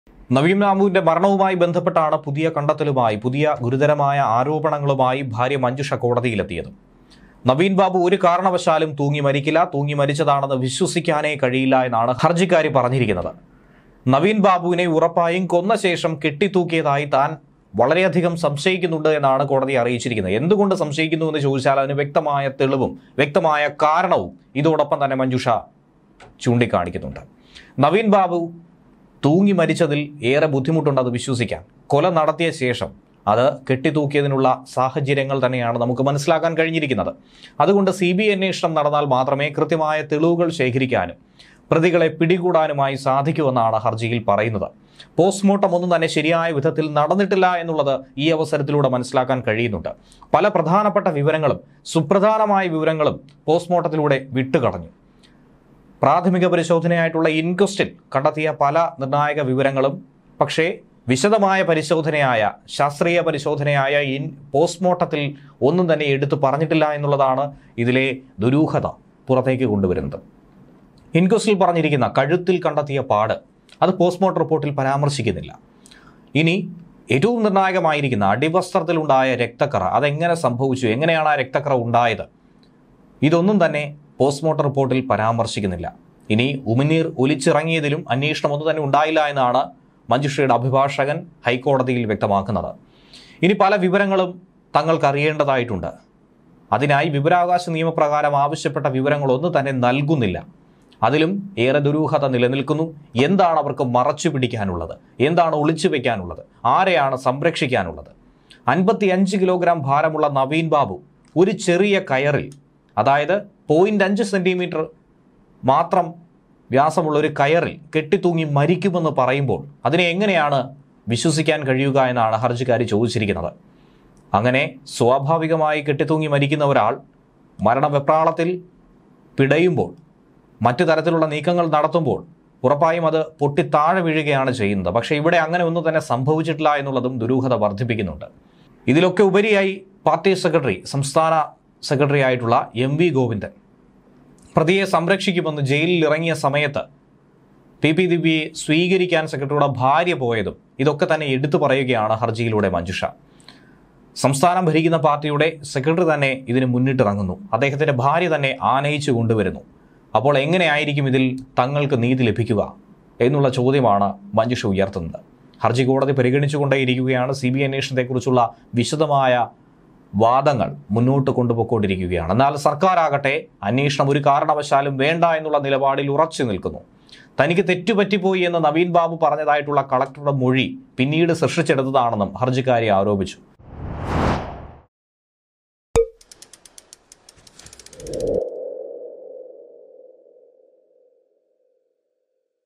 Naveen സംശയമെന്ന് ഭാര്യ കോടതിയിൽ
​ കോടതിയിൽ ബോംബ്
പ്രതികളെ പിടിക്കാൻ
മഞ്ജുഷ നേരിട്ടിറങ്ങി

@malayalivarthakerala 2024